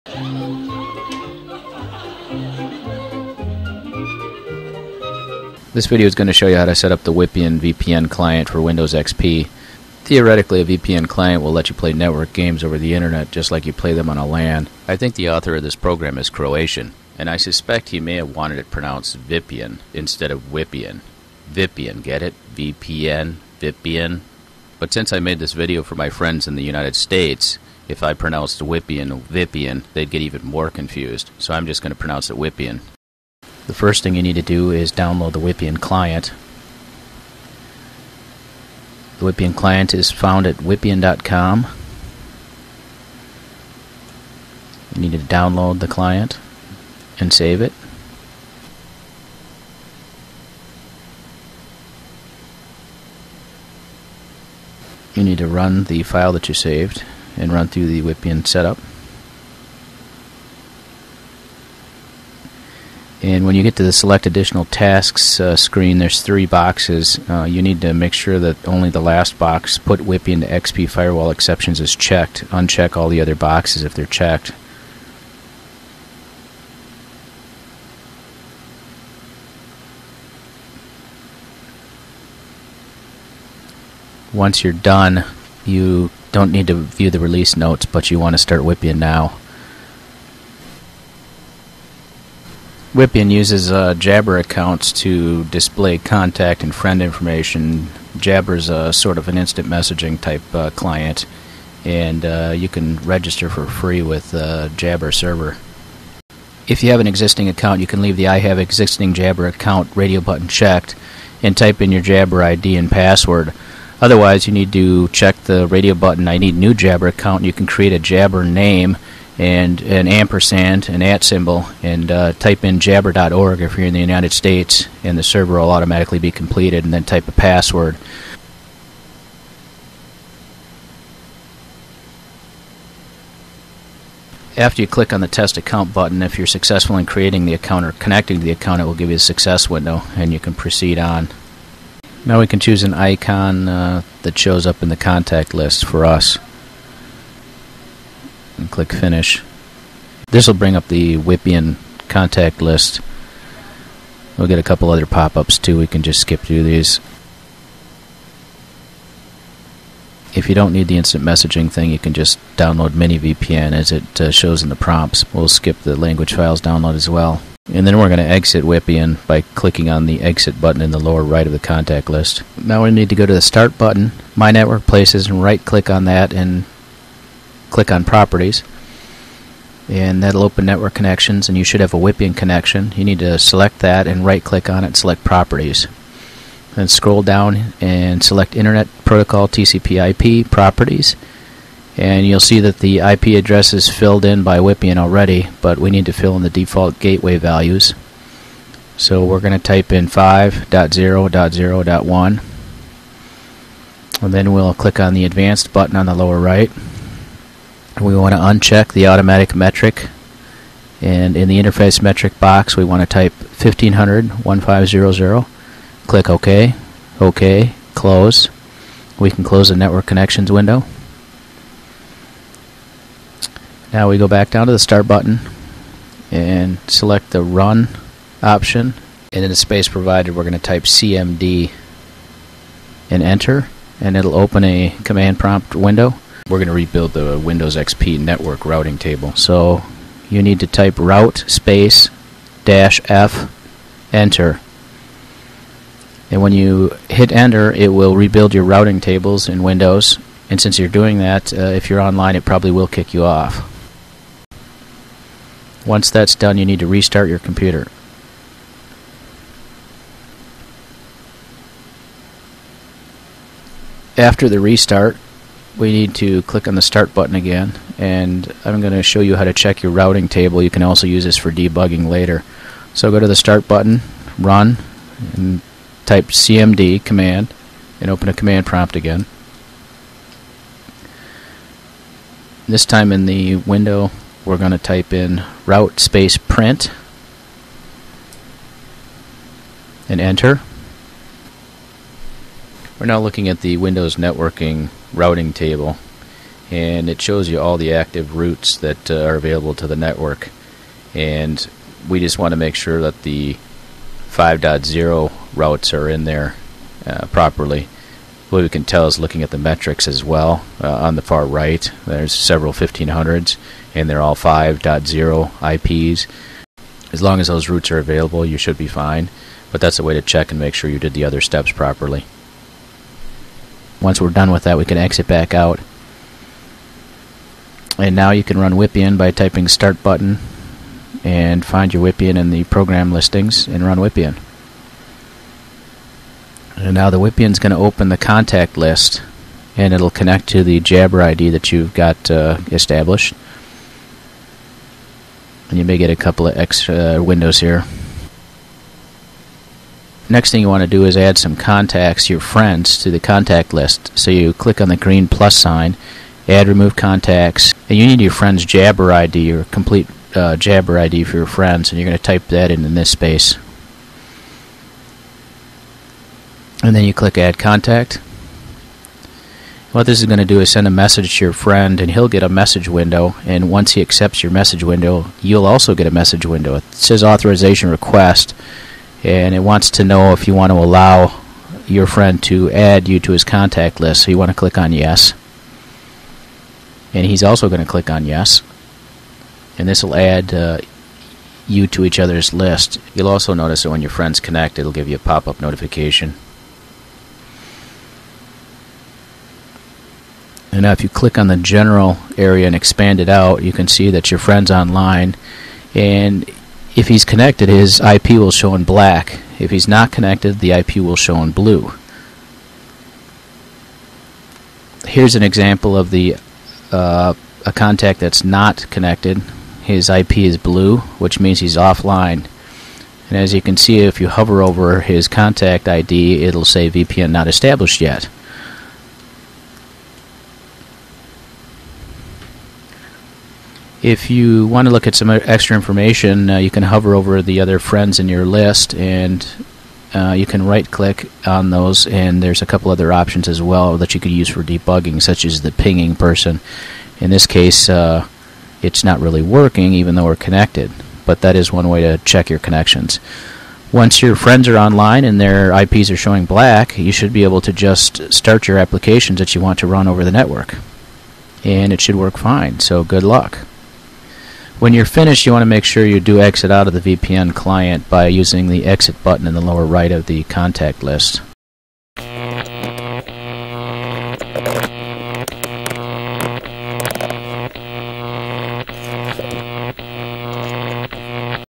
How to Set Up Wippien VPN in XP
A video on how to install and set up Wippien VPN client for Windows XP.